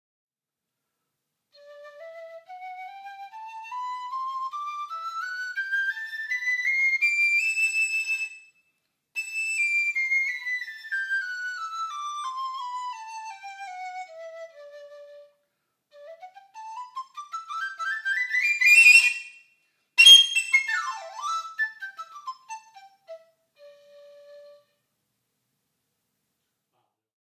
梆笛－音階示範與即興演奏 Bangdi
由香港中樂團與盧森堡小交響樂團合辨的國際作曲大賽
An international composition competition co-presented by Hong Kong Chinese Orchestra and Luxembourg Sinfonietta/Luxembourg Society for Contemporary Music.